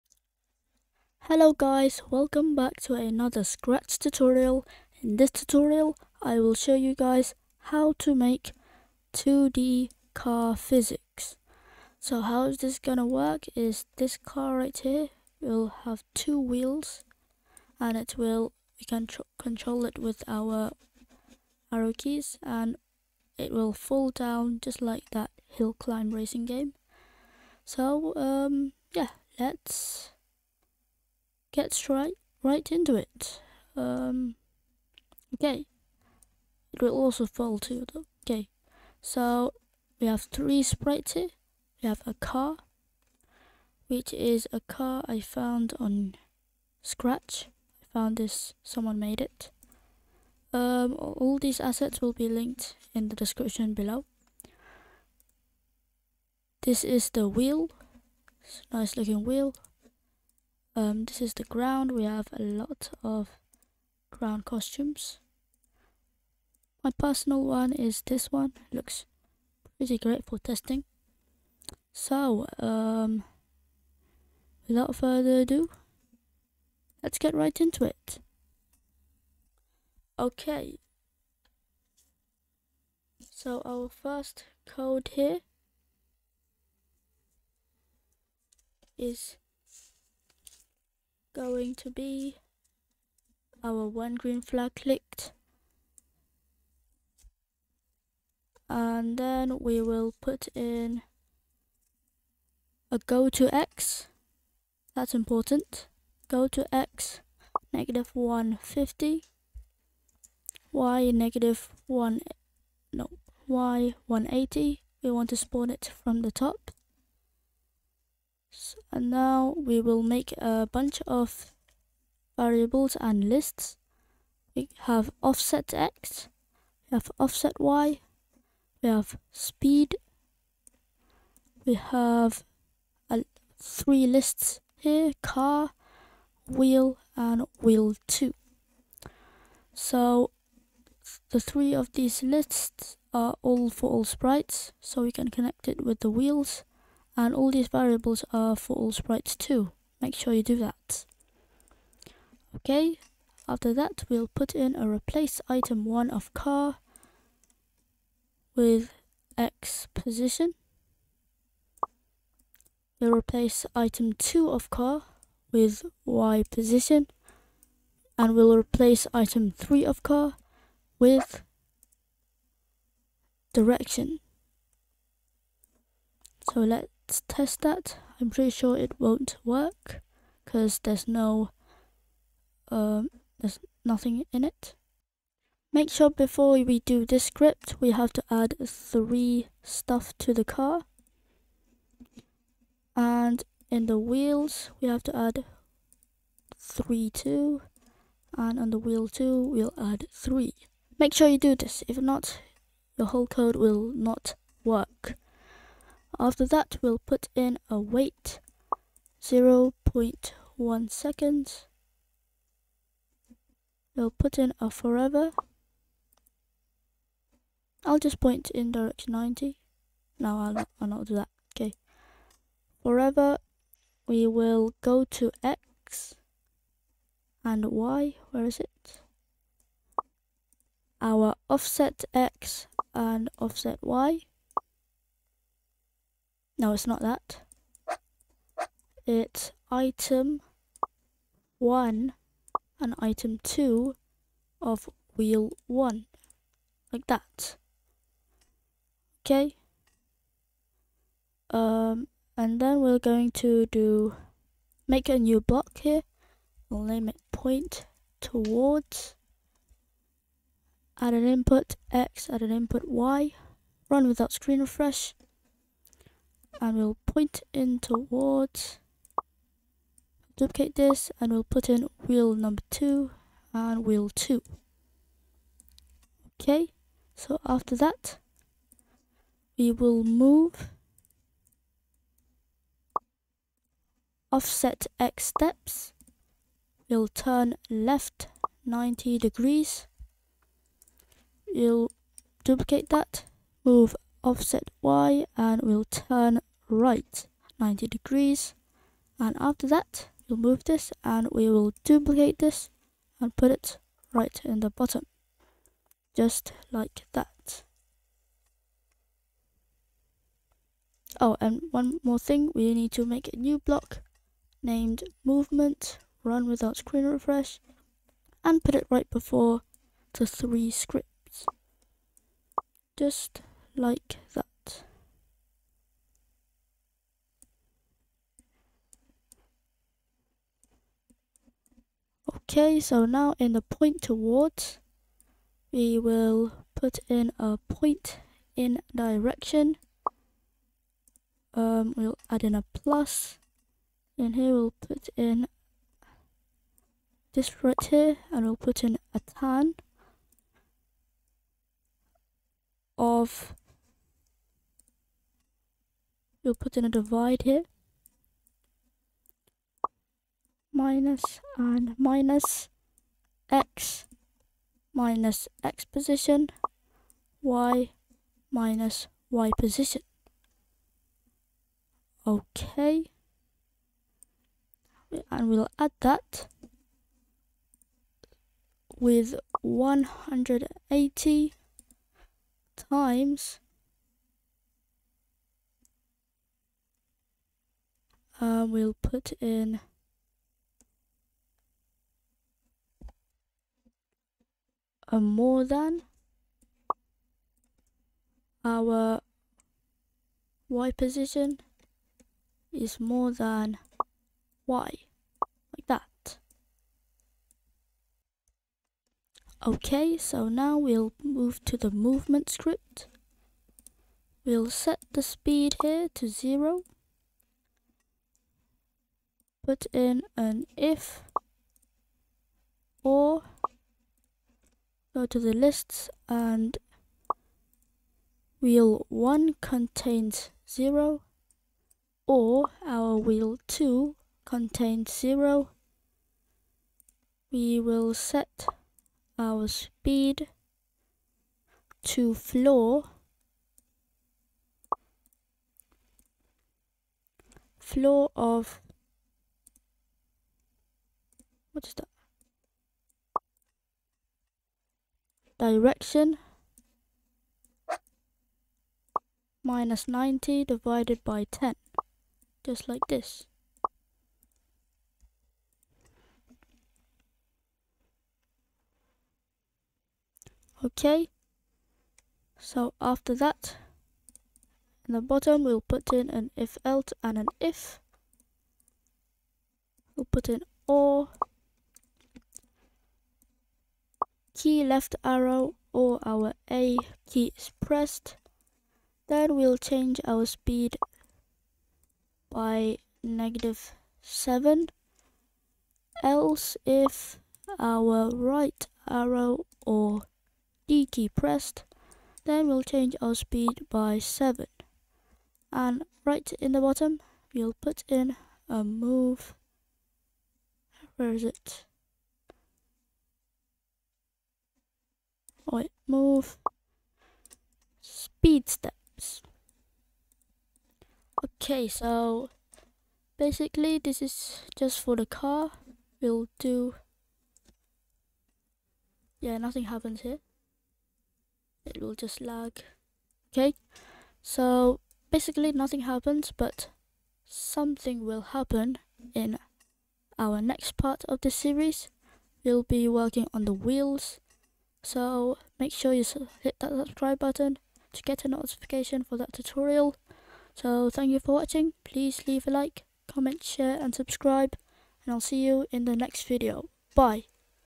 How to Make 2D Car Physics in Scratch (Part 1)
In this video, I will show you how to make 2D Car physics in Scratch!